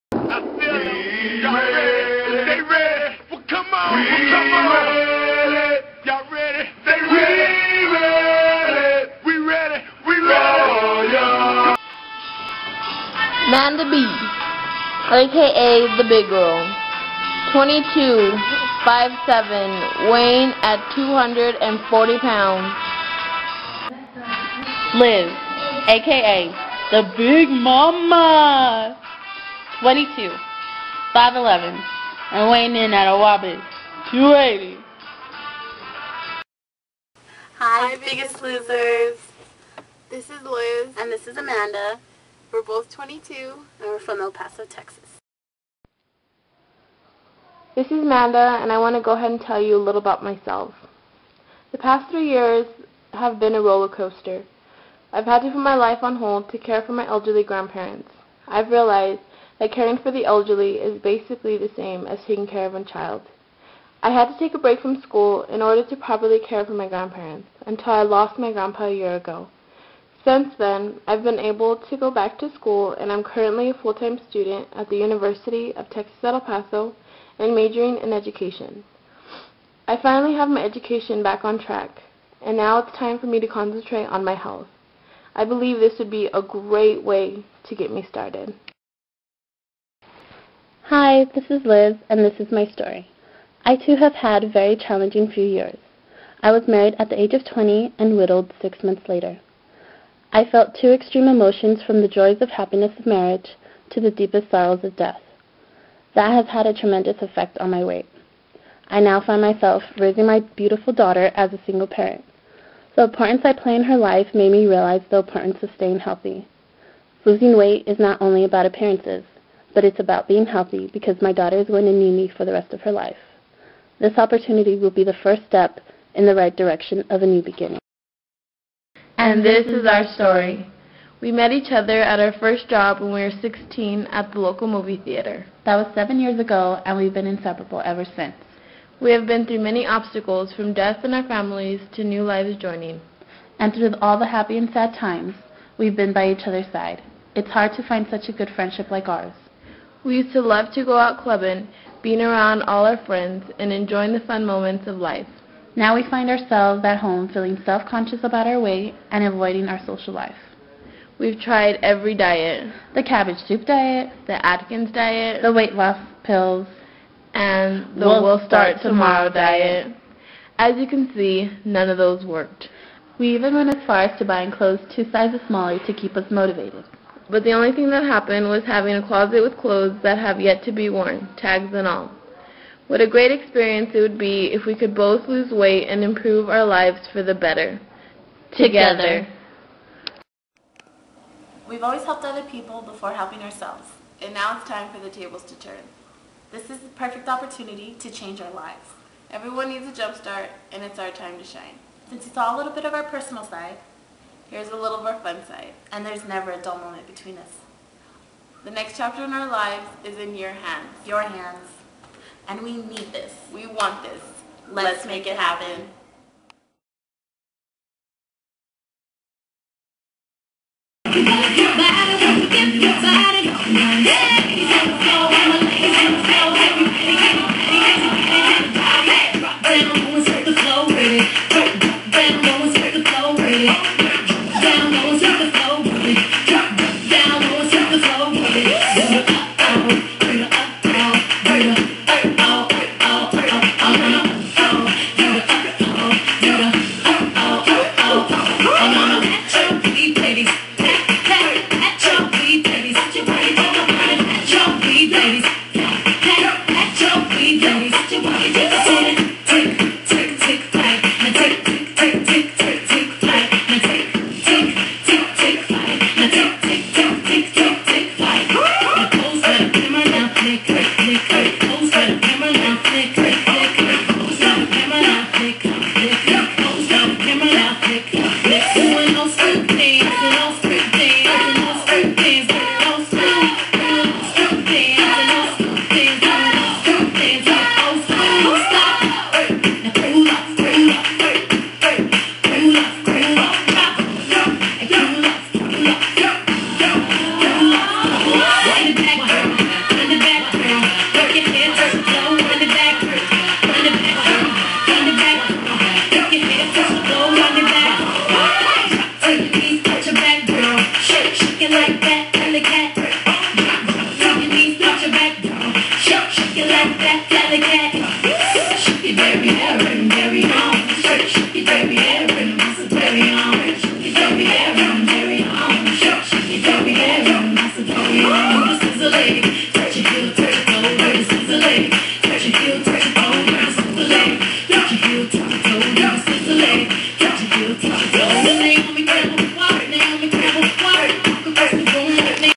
Biggest Loser 6 Audition
we submitted our video to NBC, in hoping to become the next BIGGEST LOSER!! Wish us the best of luck. .

let us know what u think! thx